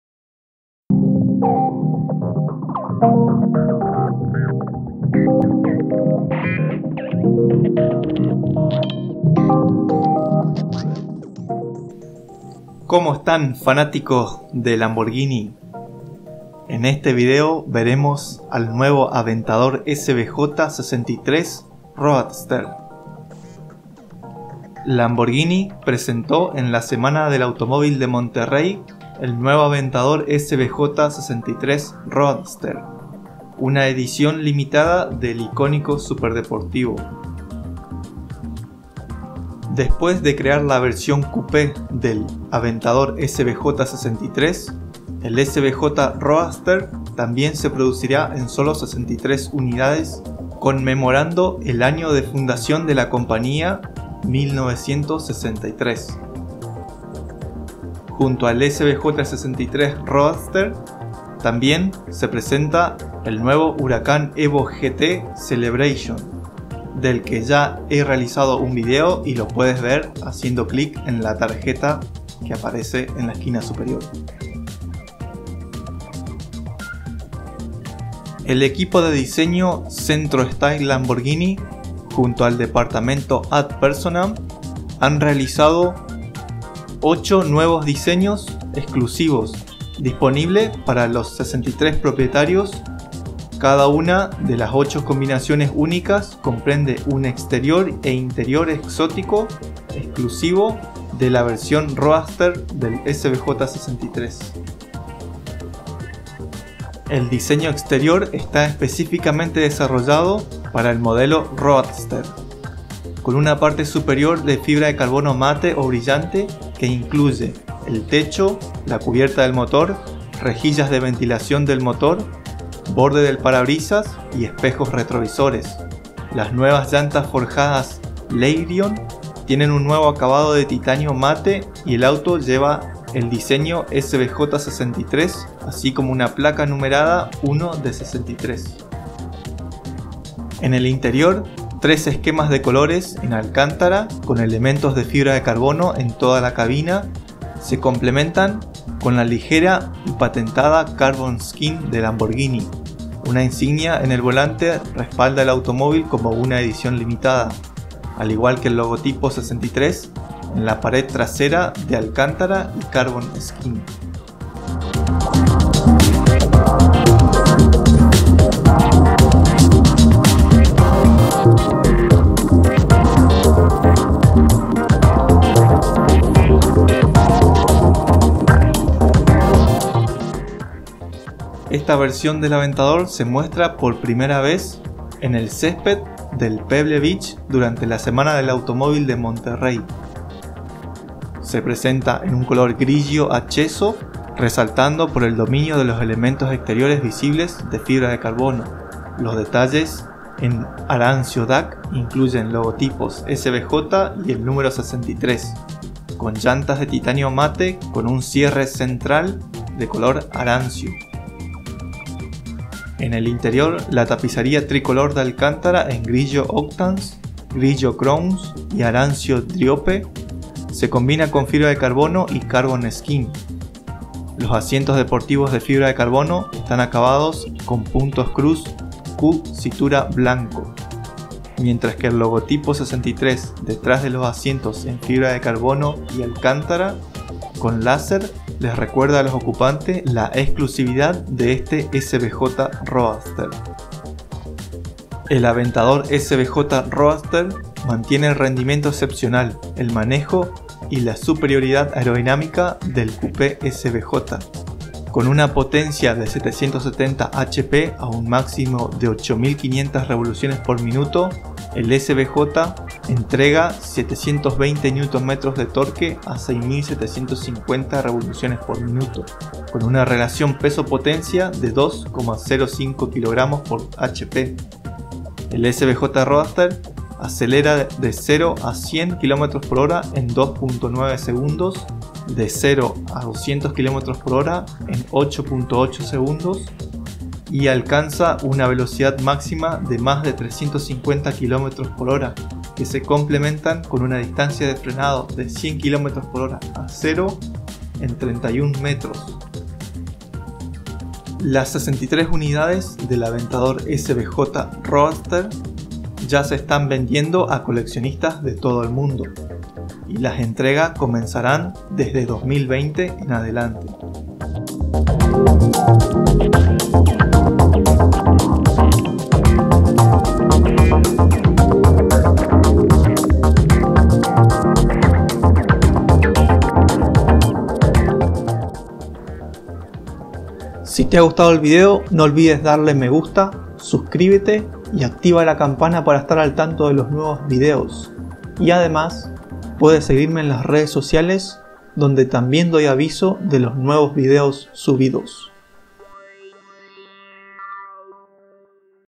2019 Lamborghini Aventador SVJ 63 Roadster | Un homenaje a la fundación de Lamborghini en 1963
Lamborghini presento el Aventador SVJ 63 Roadster, una edición limitada del icónico superdeportivo V12 en el Monterey Car Week 2019. Después de crear el exclusivo coupé SVJ 63, el SVJ 63 Roadster también se producirá en solo 63 unidades, conmemorando el año de fundación de la compañía 1963.